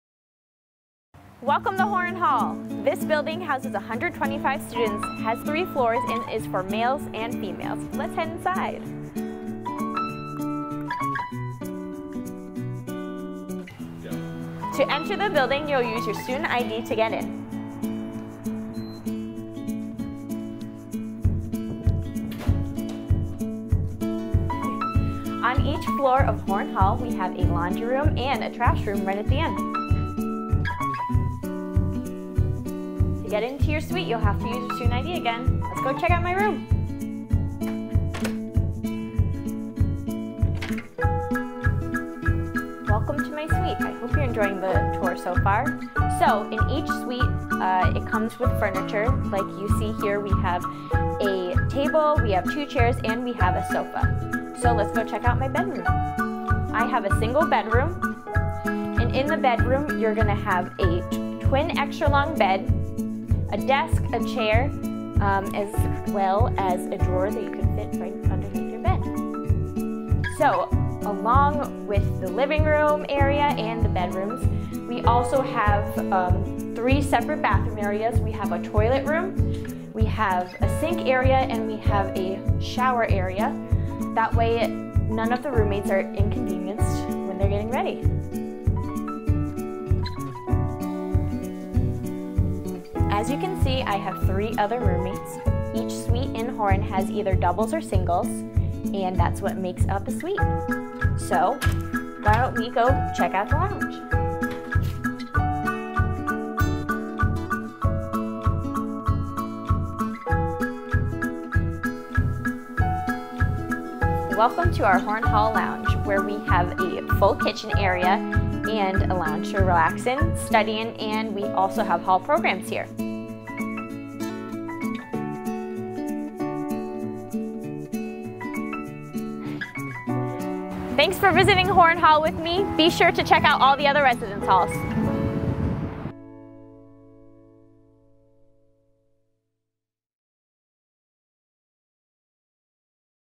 Dean College Horne Hall Tour
Check out the living space for our Horne Hall residence hall from Jackie! Opened in January 2013, Horne Hall is the newest residence hall on campus. Comprised of a total of 120 beds in a suite-style format, this residence hall is primarily for juniors and seniors. Each unit is a one- or two-bed configuration featuring a separate bathroom, washer and dryer, on every floor and state-of-the-art common room.